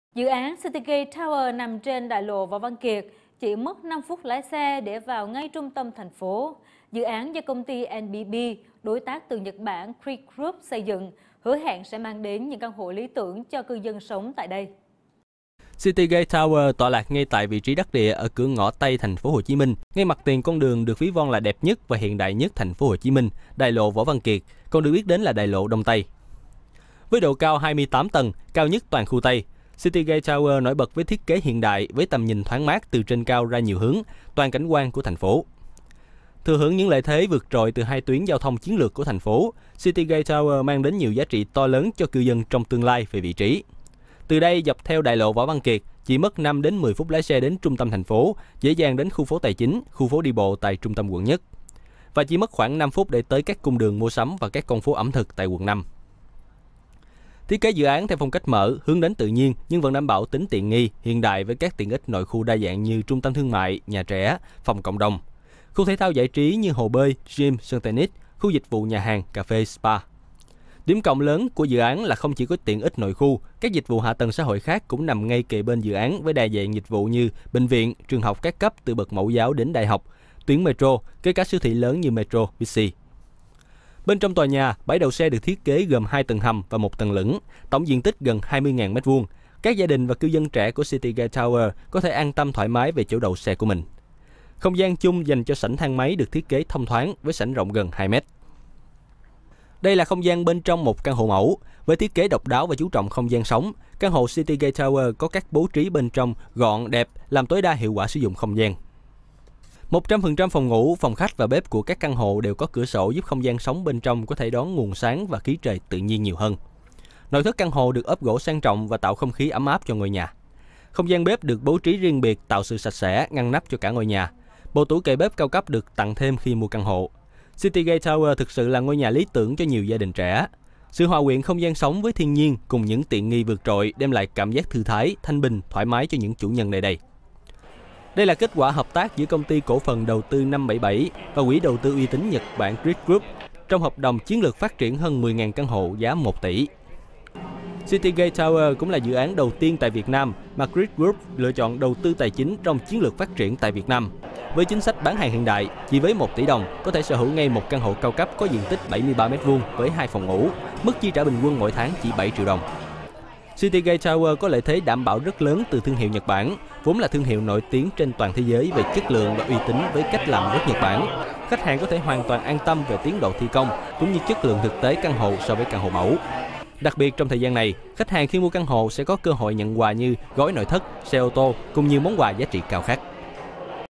FBNC - City Gate Towers: Căn hộ lý tưởng ngay trên Đại lộ Võ Văn Kiệt
Dự án City Gates Towers nằm ngay trên Đại lộ Võ Văn Kiệt, chỉ mất 5 phút lái xe đề vào ngay trung tâm thành phố. Dự án do công ty NBB đối tác từ Nhật Bản Creed Group xây dựng, hứu hẹn sẽ mang đến những căn hộ lý tưởng cho cư dân sống tại đây.